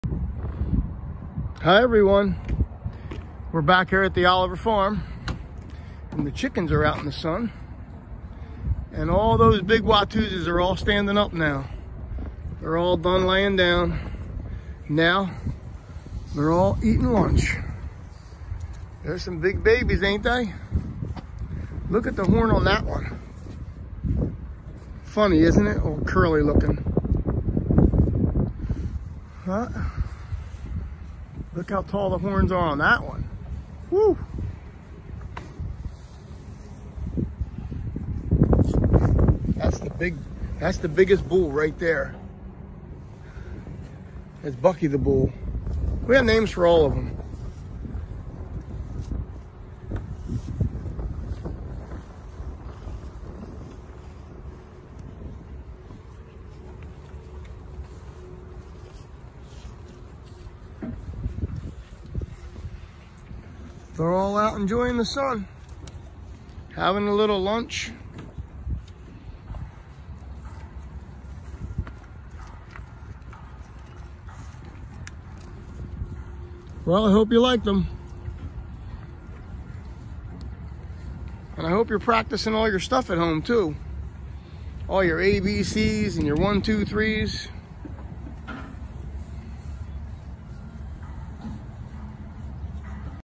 Watusi Lunchtime
These cows are soaking up the sun! Hopefully everyone can get outside and enjoy the sunshine today.